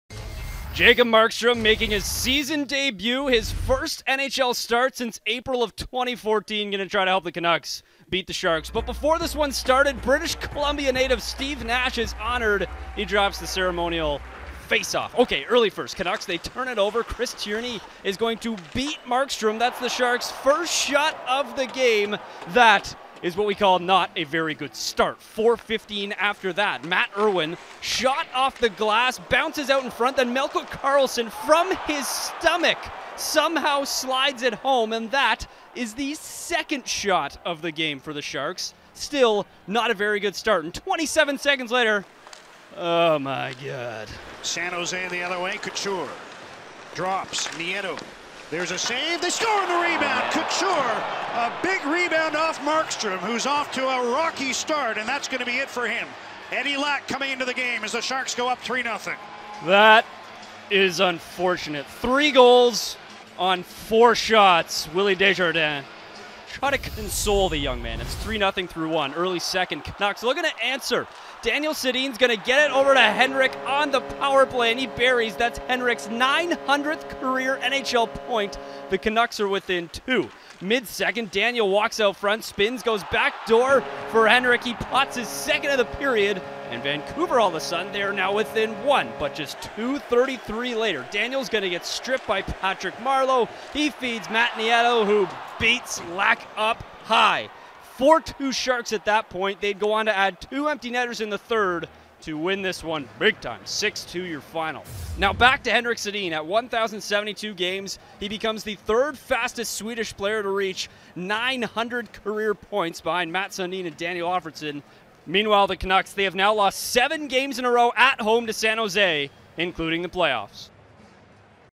Henrik Sedin gets 900th point in loss to Sharks
Henrik Sedin scored twice in Vancouver's 6-2 loss to the Sharks, getting his 900th-career point. The Sharks chased starting goalie Jacob Markstrom after he gave up three goals on four shots.